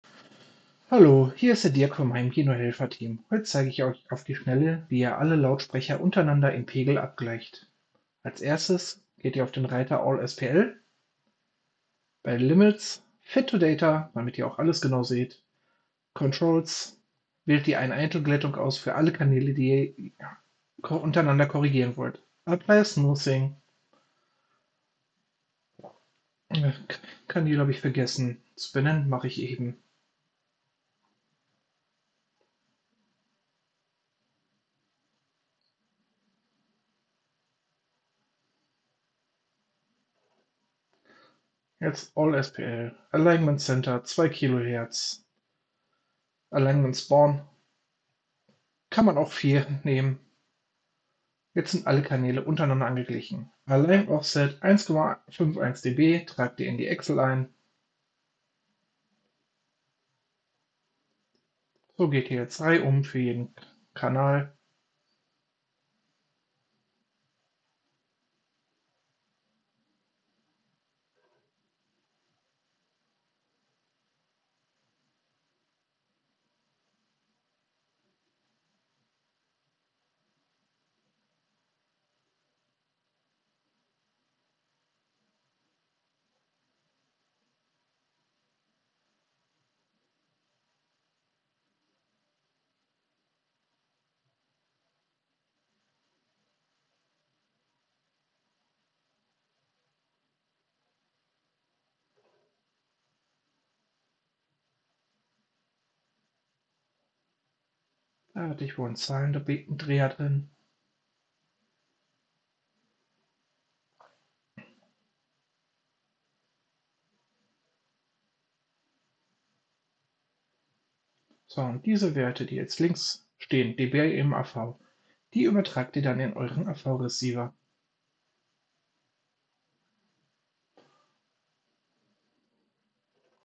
Pegelanpassung für Audyssey mit REW
Mithilfe von REW alle Pegel untereinander anpassen und dank der Excel wird nichts ins + geregelt.

gemacht

Hier findet ihr die anderen Systemübergreifenden EA REW Versionen